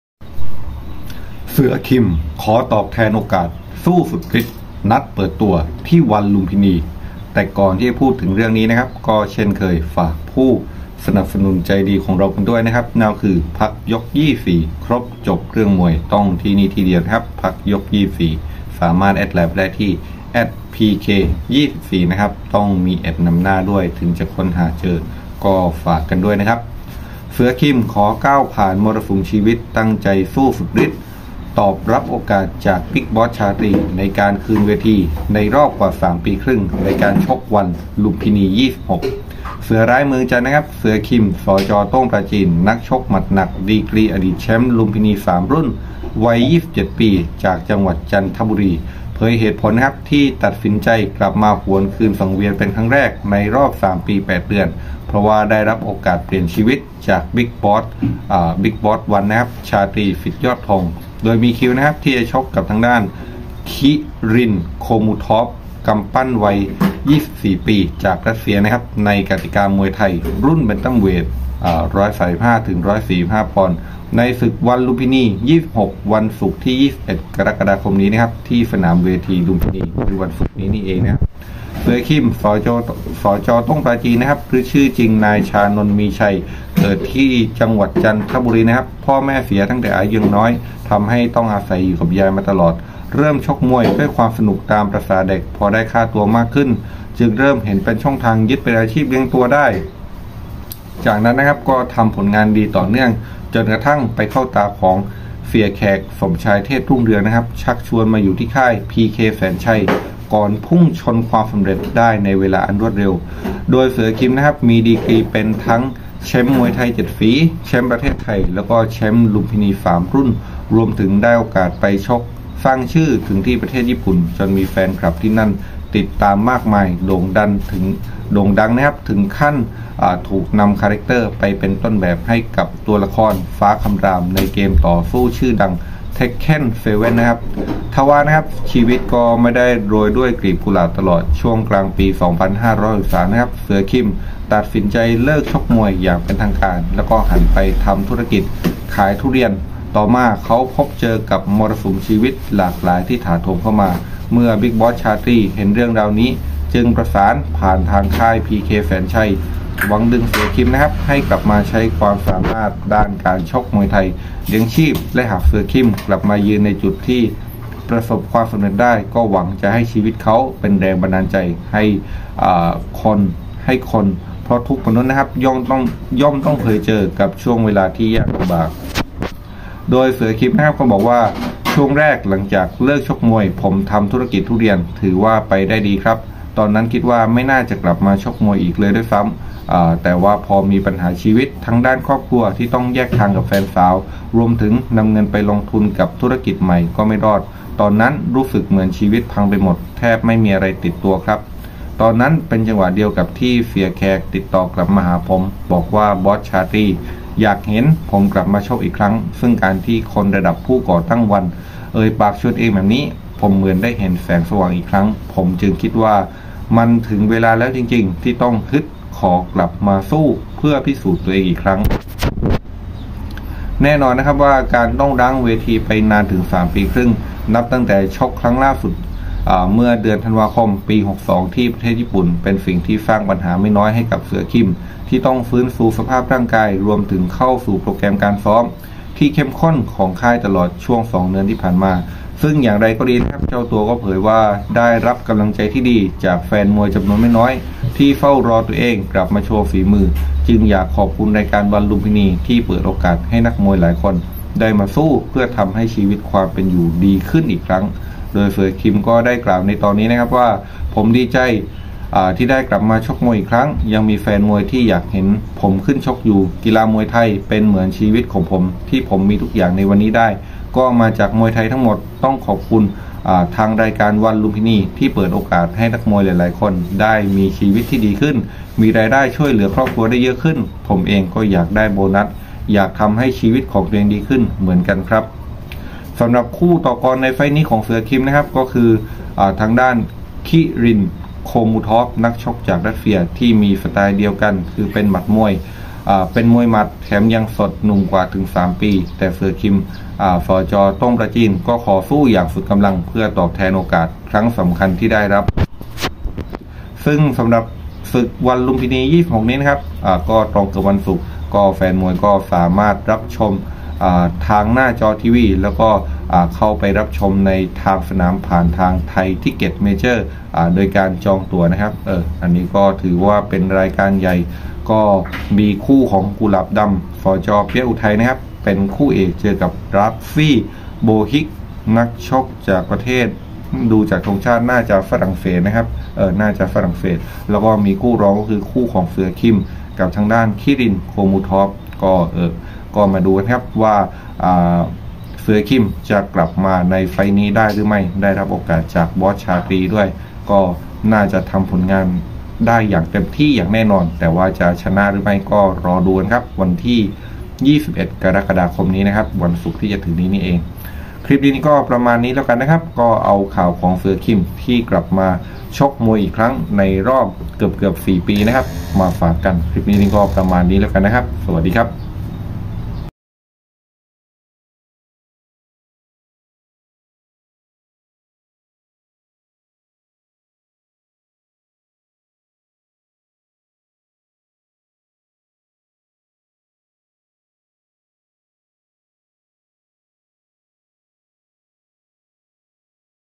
เสือคิม กลับมาชกเเล้ว
เสือคิม สจ.โต้งปราจีน กลับมาชกอีกครั้ง
ในรอบเกือบ4ปี มาครั้งนี้ขอสู้สุดใจ
เพื่อตัวเองเเละให้สมกับโอกาสที่บอส
มอบให้...